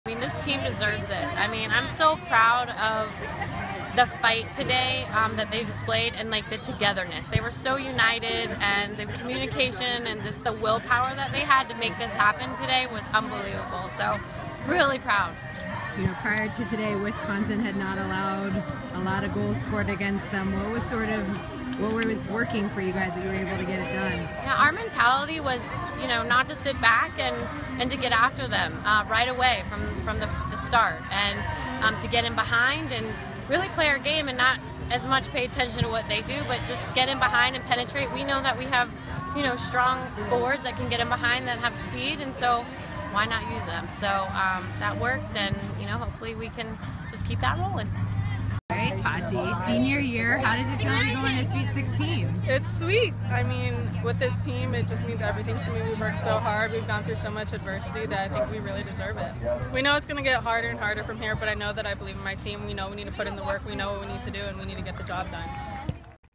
Recap: UCF women's soccer vs. Wisconsin (11-21-14)
The Knights took down fourth-seeded Wisconsin in a 3-2 win for their second Sweet 16 berth since 2011. Head coach Tiffany Roberts Sahaydak and senior forward Tatiana Coleman discuss the win.